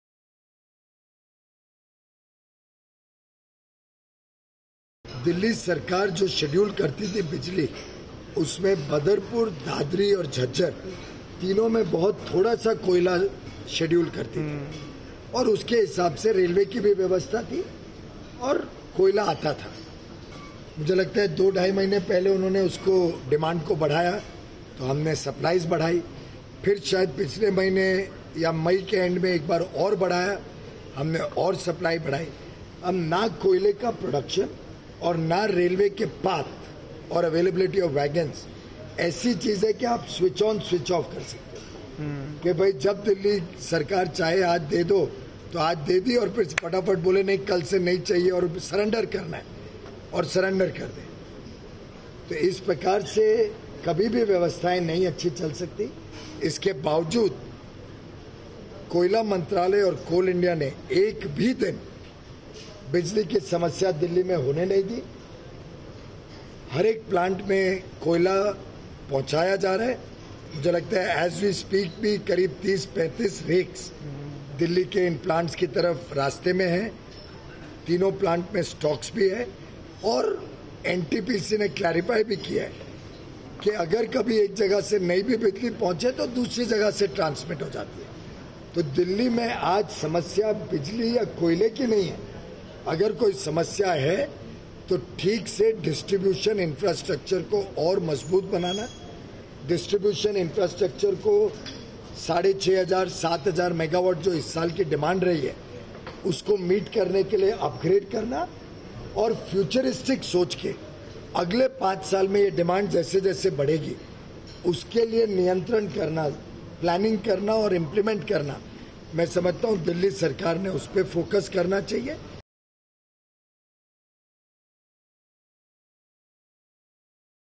No shortage of coal or electricity in Delhi: Piyush Goyal
New Delhi, Jul 05 (ANI): While briefing media in the national capital, Union Minister Piyush Goyal ruled out Aam Aadmi Party’s coal stock crisis at Delhi-based thermal power plants and urged the Arvind Kejriwal-led government to focus on strengthening distribution infrastructure to meet electricity demand for future. He said, “There is no shortage of coal or electricity in Delhi. State government should focus on strengthening distribution infrastructure and implement plans to meet demands of future, or else the situation will stay the same for the forthcoming governments too”.